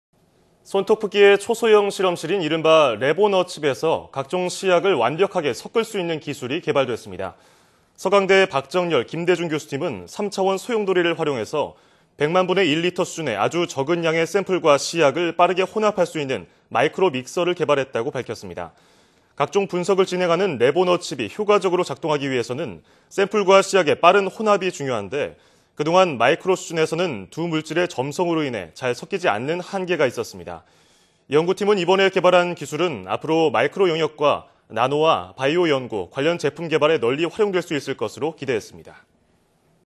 초미세량 시약 혼합 '마이크로 믹서' 개발 / YTN 사이언스
손톱 크기의 초소형 실험실인 이른바 '랩 온 어 칩'에서 각종 시약을 완벽하게 섞을 수 있는 기술이 개발됐습니다.

서강대 박정열·김대중 교수팀은 3차원 소용돌이를 활용해, 100만 분의 1리터 수준의 아주 적은 양의 샘플과 시약을 빠르게 혼합할 수 있는 '마이크로 믹서'를 개발했다고 밝혔습니다.

각종 분석을 진행하는 랩 온 어 칩이 효과적으로 작동하기 위해서는 샘플과 시약의 빠른 혼합이 중요한데, 그동안 마이크로 수준에서는 두 물질의 점성으로 인해 잘 섞이지 않는 한계가 있었습니다.

연구팀은 이번에 개발한 기술은 앞으로 마이크로 영역의 나노와 바이오 연구, 관련 제품 개발에 널리 활용될 수 있을 것으로 기대했습니다.